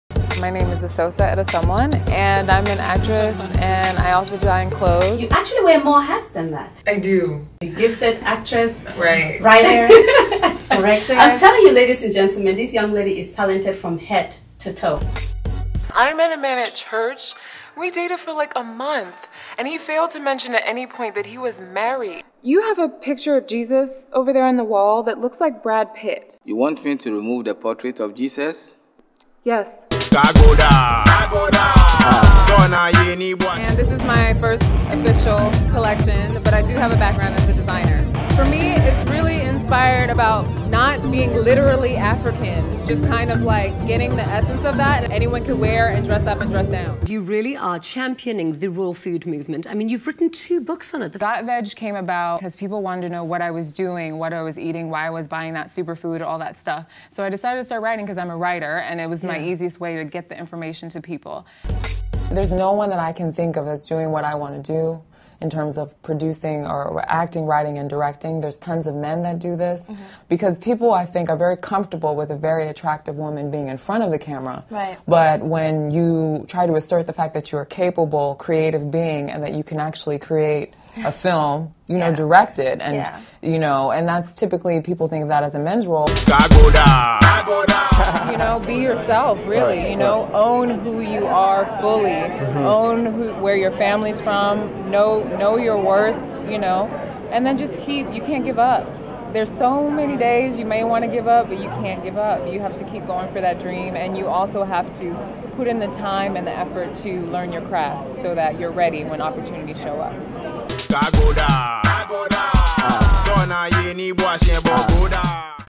Who is Esosa E?
Comprehensive Reel for Producer, Actress, Health Expert & Fashion Designer, Esosa E.